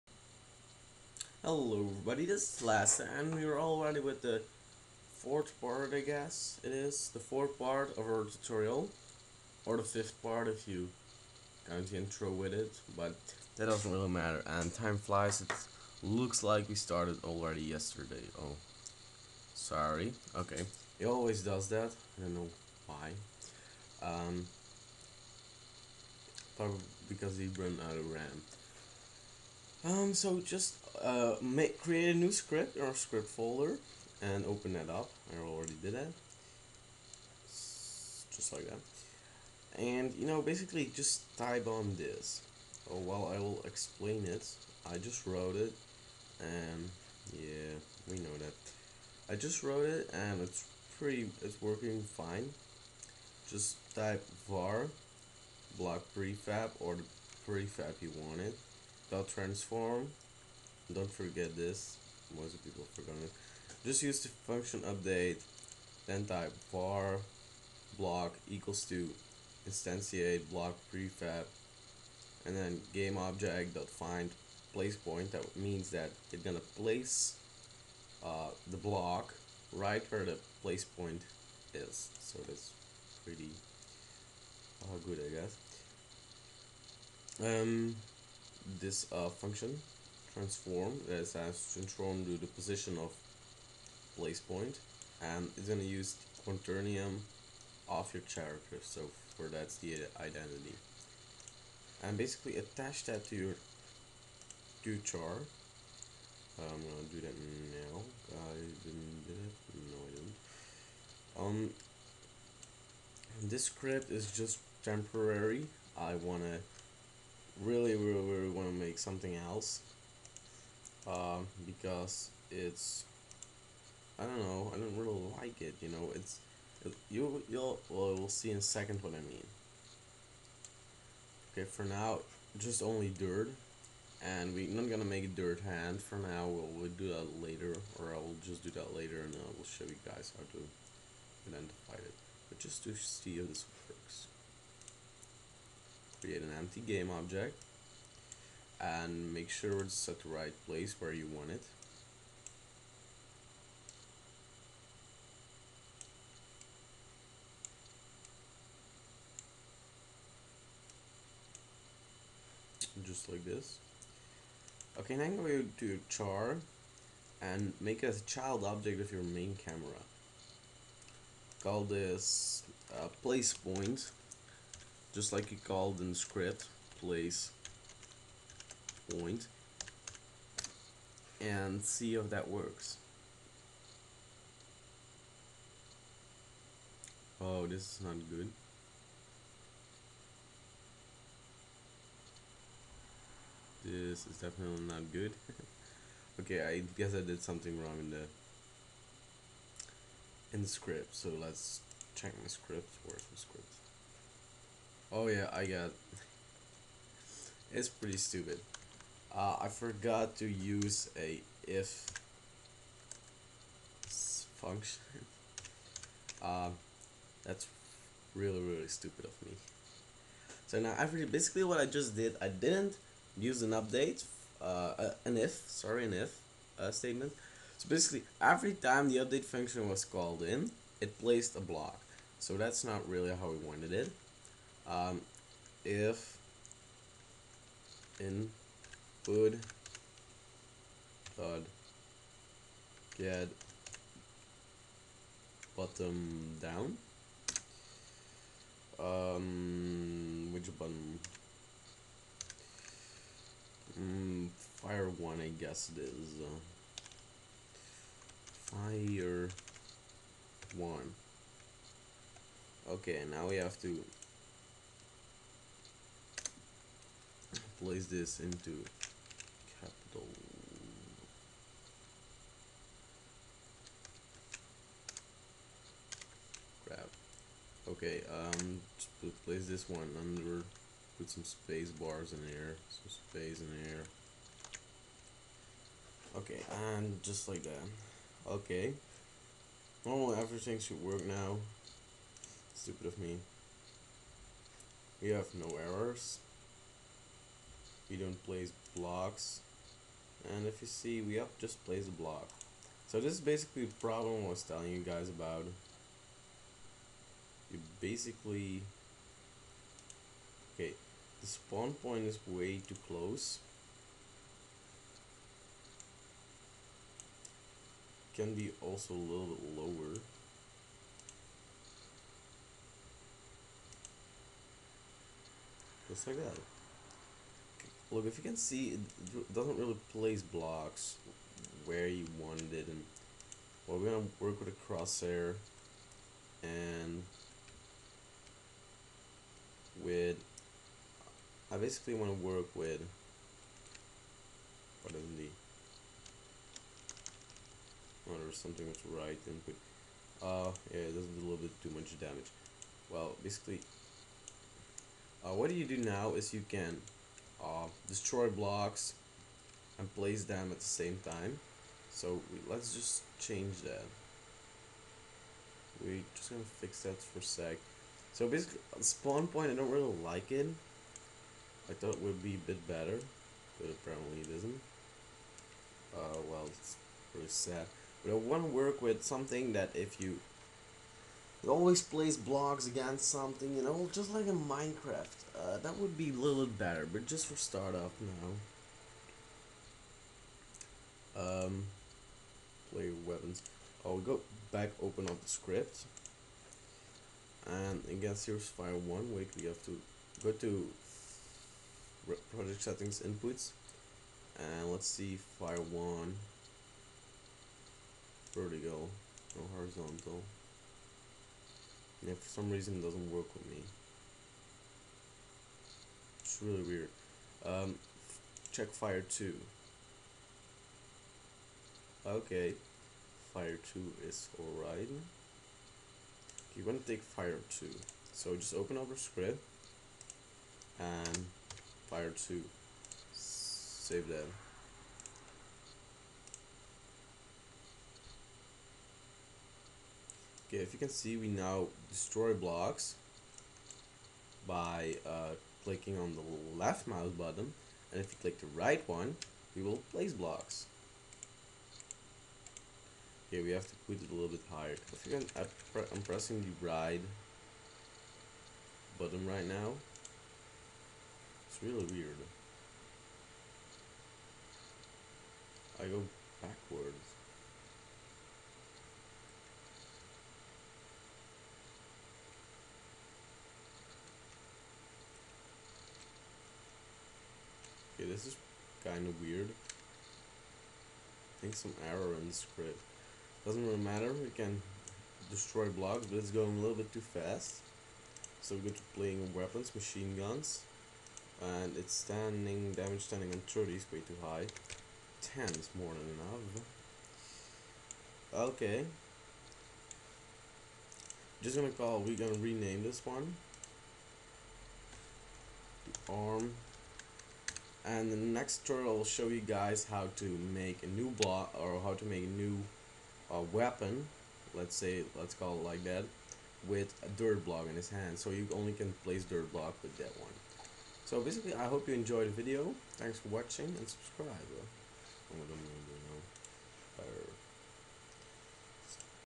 11. Unity 3D Minecraft tutorial arms and placing blocks
Today we are gonna ad arm and begining whit the placing block code